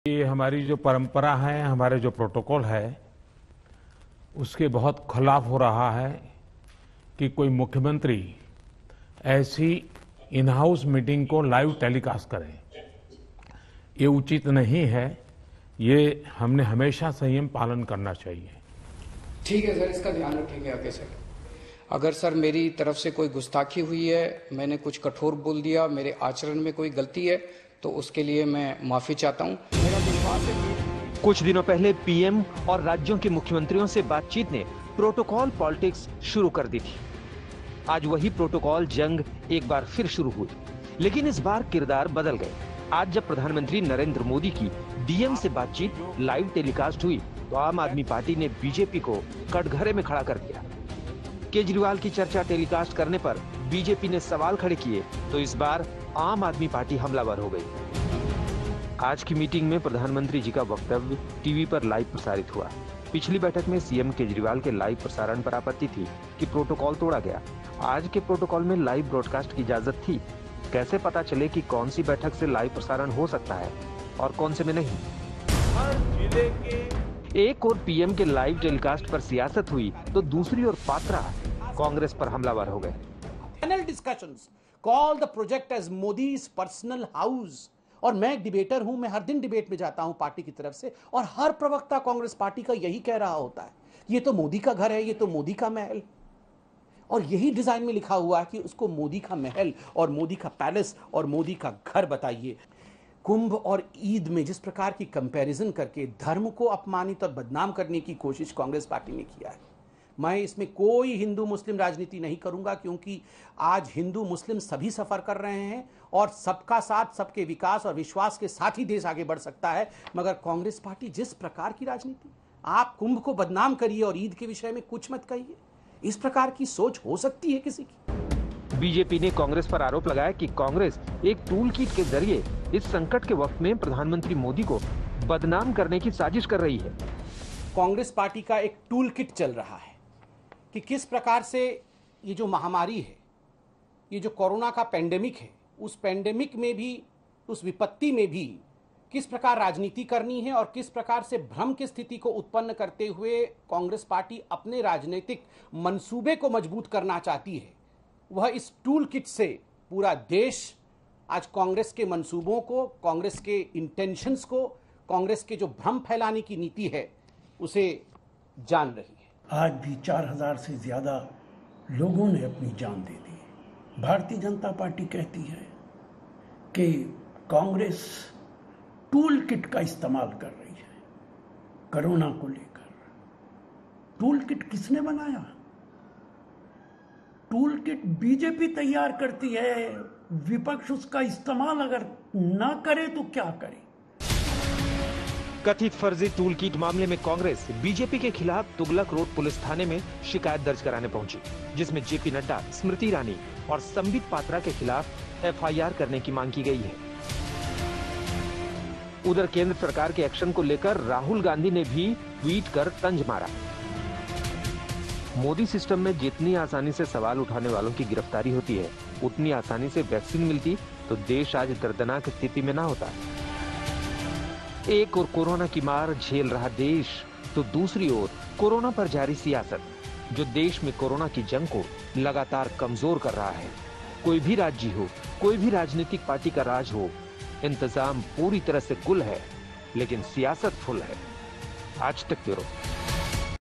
प्रोटोकॉल पर बवाल, टेलीकास्ट पर सवाल, Corona में इंतजाम गुल.. सियासत फुल!
लकिट को लेकर बीजेपी की तरफ से कांग्रेस पर लगाए गए आरोप का मामला अब तूल पकड़ता नजर आ रहा है. जुबानी जंग के बाद अब यह मामला पुलिस थाने जा पहुंचा है. कांग्रेस  ने बीजेपी अध्यक्ष  जेपी नड्डा और पार्टी के प्रवक्ता संबित पात्रा समेत कई बीजेपी नेताओं के खिलाफ दिल्ली पुलिस में एफआईआर दर्ज करने की मांग की.